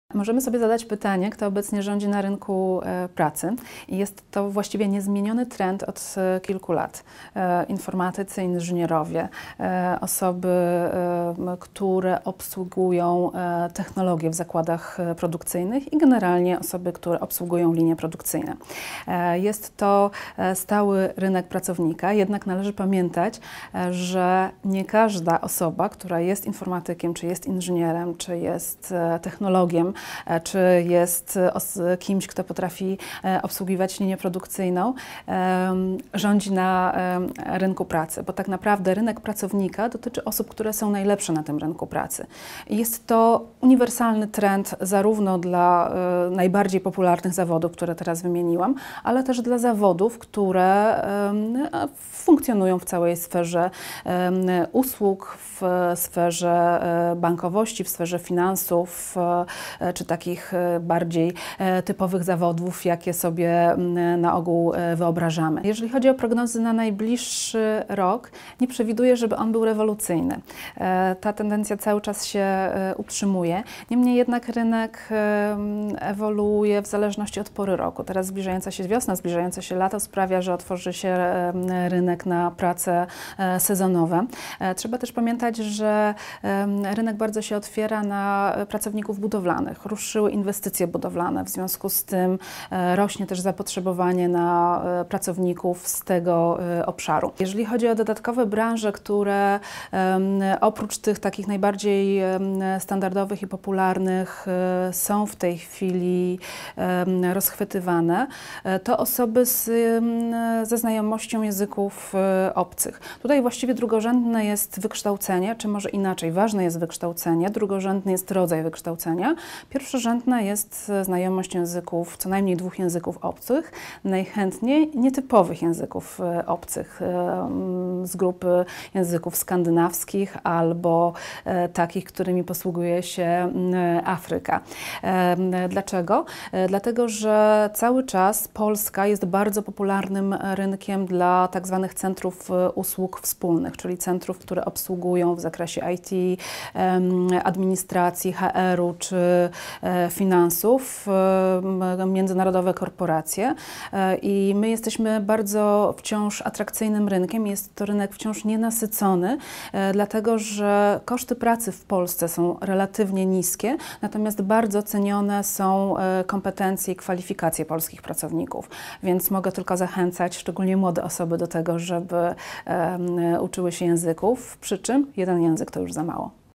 Kto zawsze znajdzie pracę?
Wypowiedź: Lucyna Pleśniar, Prezes Zarządu agencji rekrutacyjnej PEOPLE.

W niektórych branżach już mamy rynek pracownika. Przykład? Informatycy, inżynierowie, wyspecjalizowani pracownicy obsługujący technologie w zakładach i obsługujący linie produkcyjne – to osoby, które są najczęściej poszukiwane przez firmy. Chociaż ofert pracy jest dla nich dużo to okazuje się, że na etat mogą liczyć tylko nieliczni. Kto jest najbardziej poszukiwany na rynku pracy?